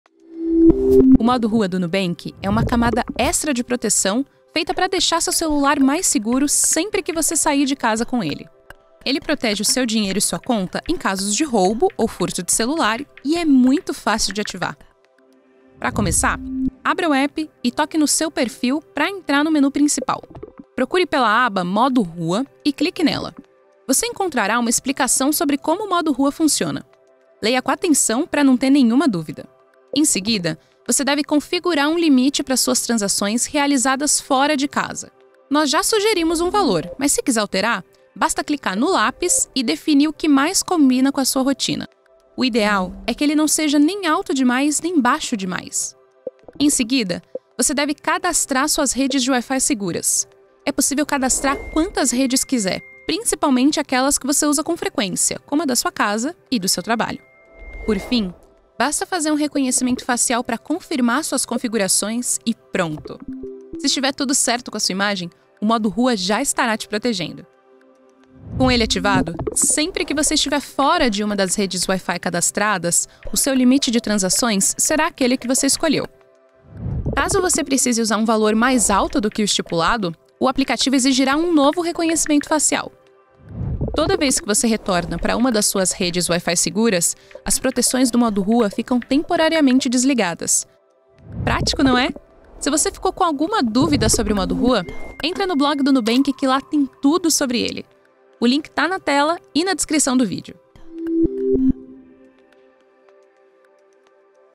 Modo Rua do Nubank: passo a passo para usar | Nu Apresenta
O Modo Rua é uma camada extra de proteção do app do Nubank. Ele garante a segurança do seu dinheiro e da sua conta em situação de roubo ou furto de celular.

Ativar o Modo Rua no seu app é simples e rápido – e neste vídeo, nós te mostramos como fazer isso. Descubra:
0:30 - Como definir o limite do Modo Rua;
0:47 - Como cadastrar a rede Wi-Fi do Modo Rua.

Quer saber mais? Então acesse o nosso blog